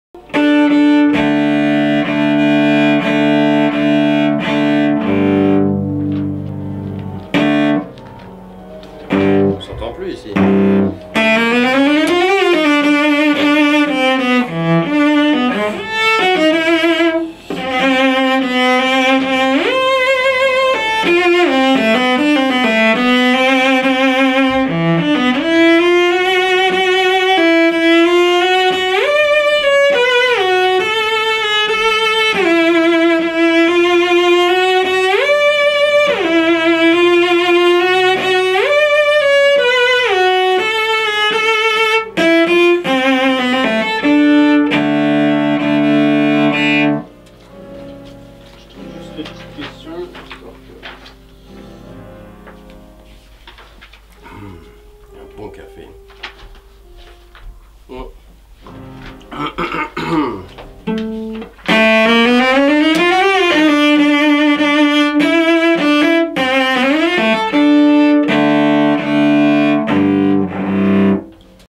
Gautier Capucon rehearses in his dressing room
Gautier Capucon repete dans sa loge avant de repeter avec Martha Argerich pour son concert lors du Verbier Festival 2011. Cette video est tournée juste avant son wallcast.